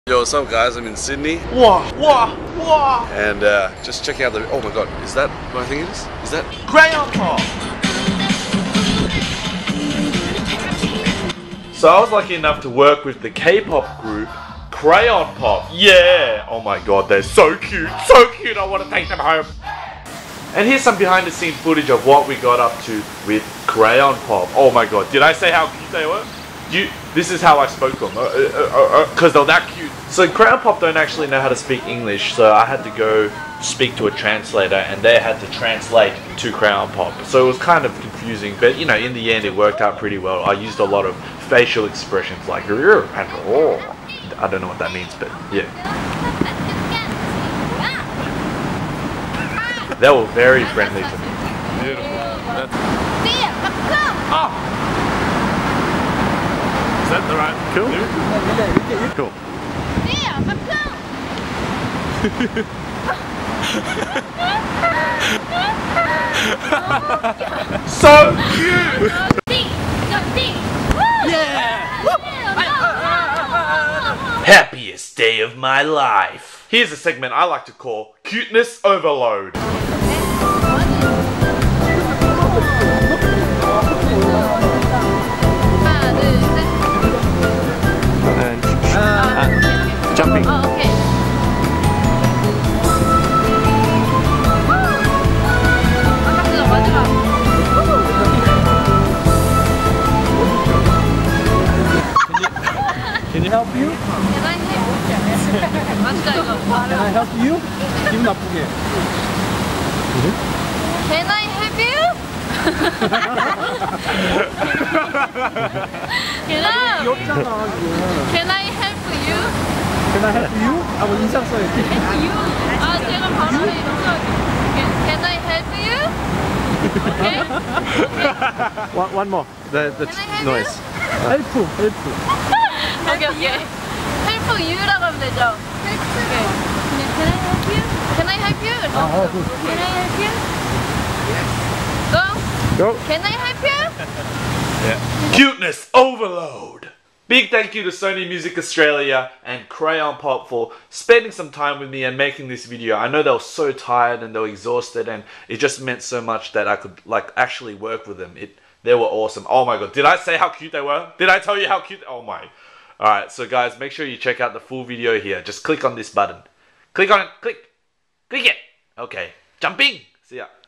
Working with CRAYON POP! - vlog #83
Just some behind the scenes footage with Crayon Pop!
They're so CUTE! did I already tell you that? hahaha CUTE CUTE

take care and sleep well Lol